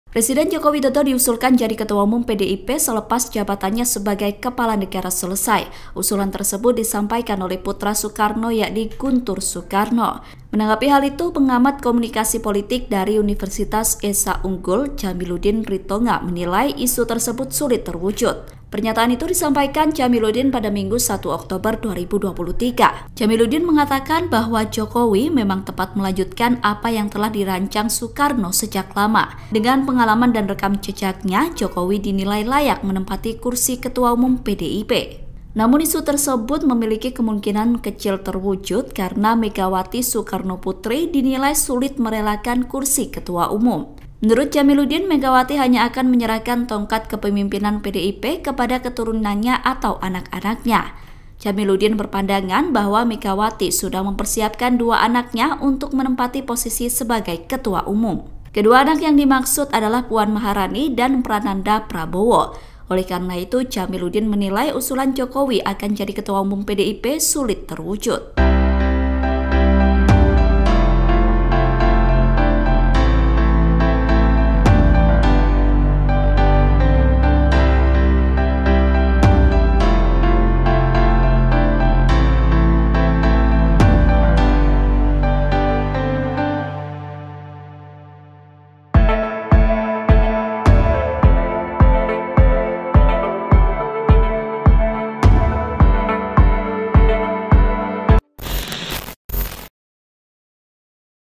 Jokowi Diusulkan Jadi Ketum PDIP Lengserkan Megawati Pasca Jabatannya Habis, Pengamat: Sulit Terjadi
TRIBUN-VIDEO.COM - Presiden Joko Widodo diusulkan jadi Ketua Umum PDIP selepas jabatannya sebagai kepala negara selesai.

Usulan tersebut disampaikan oleh putra Soekarno, Guntur Soekarno.

Menanggapi hal itu, Pengamat Komunikasi Politik dari Universitas Esa Unggul Jamiluddin Ritonga menilai isu tersebut sulit terwujud.

Pernyataan itu disampaikan Jamiluddin Minggu (1/10/2023).

Jamiludin mengatakan bahwa Jokowi memang tepat melanjutkan apa yang telah dirancang Soekarno sejak lama.

Dengan pengalaman dan rekam jejaknya, Jokowi dinilai layak menempati kursi Ketua Umum PDIP.

Namun isu itu memiliki kemungkinan kecil terwujud karena Megawati Soekarnoputri dinilai sulit merelakan kursi ketua umum.

Menurut Jamiludin, Megawati hanya akan menyerahkan tongkat kepemimpinan PDIP kepada keturunannya atau anak-anaknya.

"Masalahnya, apakah Megawati mau menyerahkan tongkat estafet kepemimpinan PDIP kepada Jokowi ? Hal itu kemungkinan sangat kecil mengingat Megawati tampaknya sudah mempersiapkan anaknya untuk menggantikannya," kata Jamiluddin.

Jamiluddin berpandangan bahwa Megawati sudah mempersiapkan dua anaknya untuk menempati posisi sebagai ketua umum.

Kedua anak yang dimaksud adalah Puan Maharani dan Prananda Prabowo.

Oleh karena itu, Jamiluddin menilai usulan Jokowi akan jadi Ketum PDIP akan sulit terwujud. (Tribun-video.com/Tribunnews.com)

Artikel ini telah tayang di Tribunnews.com dengan judul Pengamat Nilai Usulan Guntur Soekarno soal Jokowi jadi Ketua Umum PDIP, Kecil Kemungkinan Terjadi,
Editor: Wahyu Aji